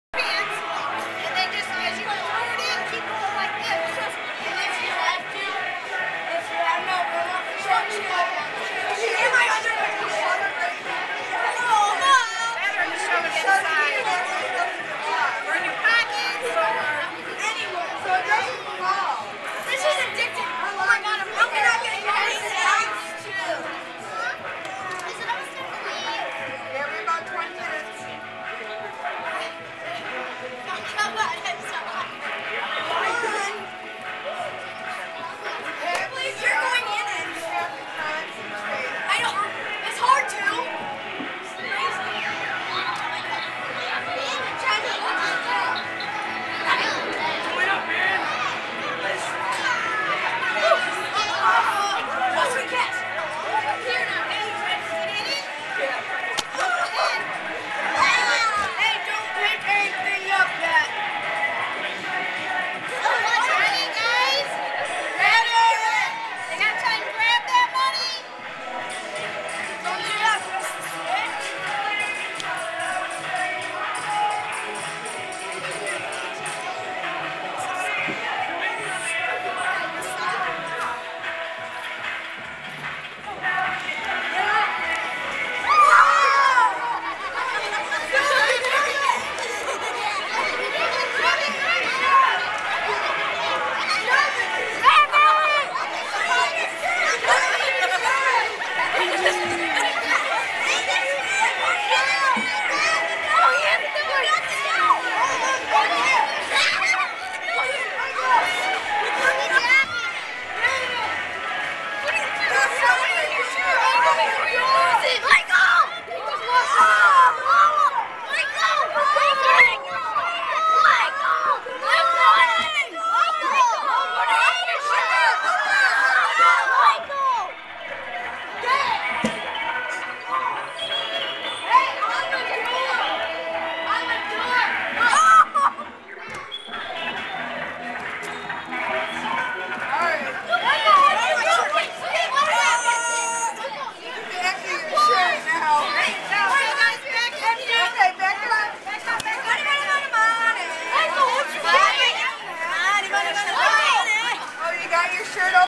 The Money Machine
A Birthday Child going into the Money Machine.  On September 7th, 2008.  At Rollhaven Skating Center in Flushing, MI.